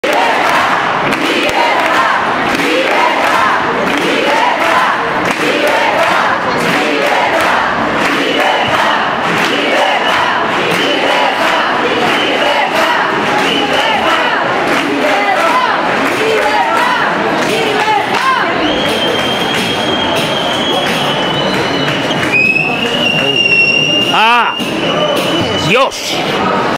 Bilbao contra el pasaporte COVID. 26 Diciembre 2021 ( 7 )
Manifestación de Bizitza contra el pasaporte Covid 26 Diciembre 2021.